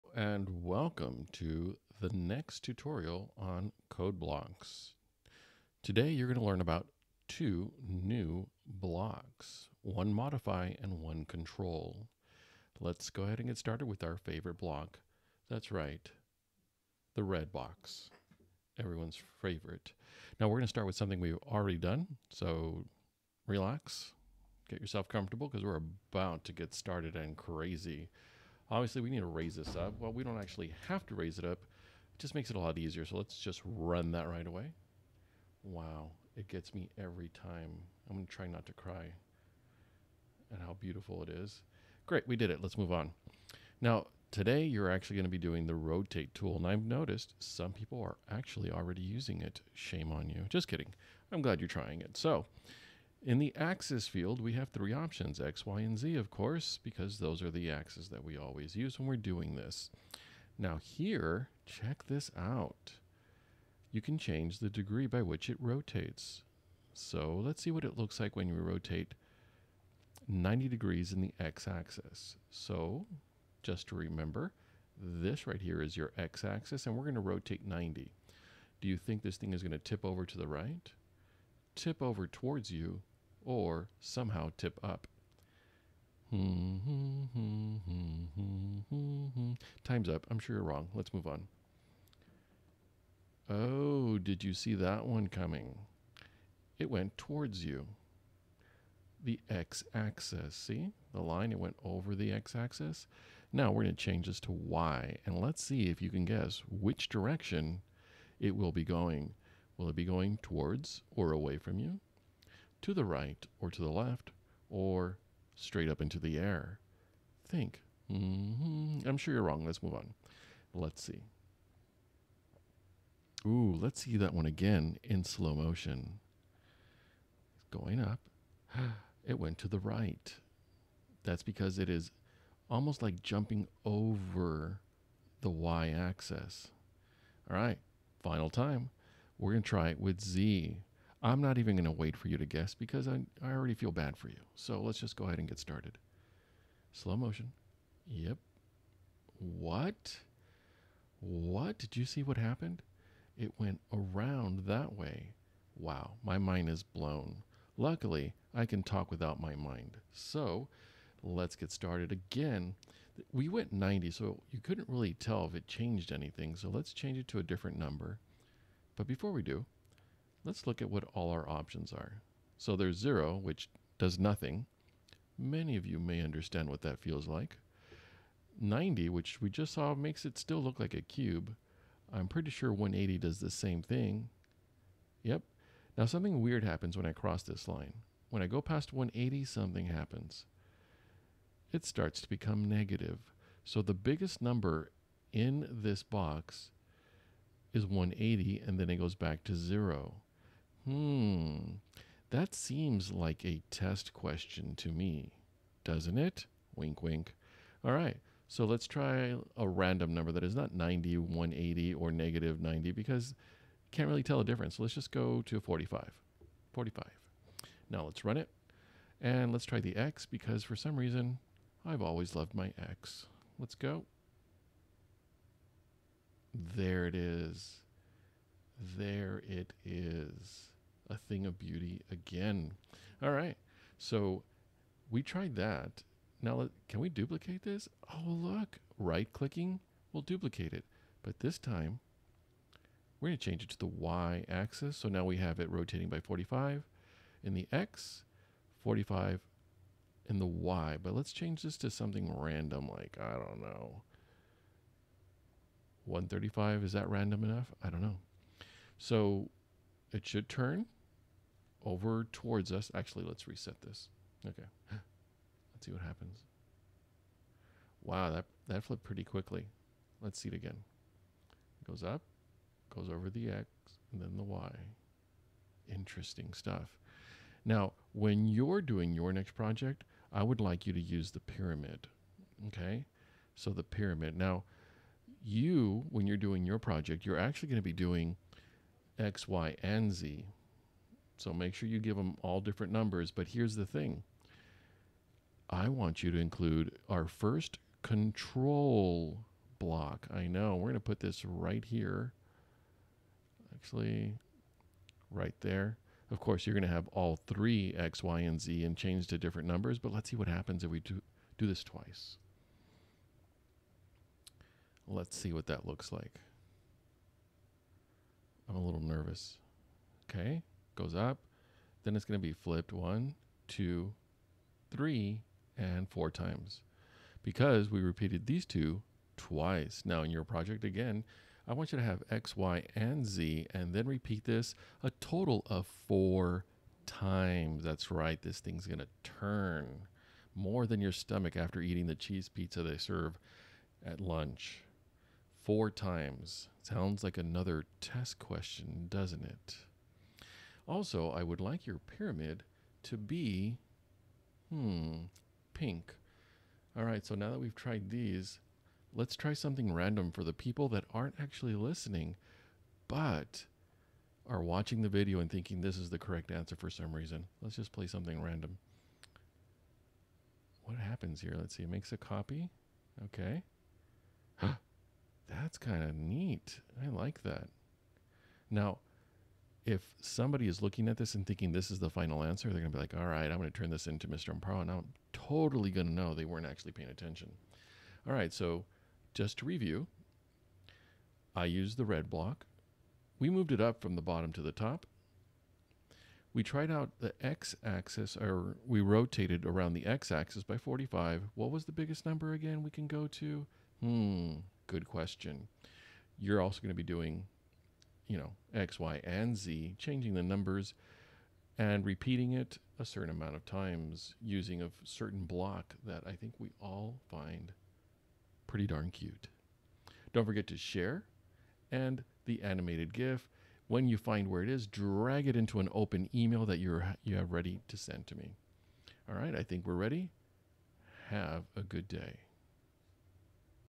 CodeBlocks 3 Introducing 2 new blocks Rotate and Repeat
In this short tutorial students will learn about two new blocks in the Codeblocks software.

Tri-color filament
Clear Glitter Filaments
Multicolor Filament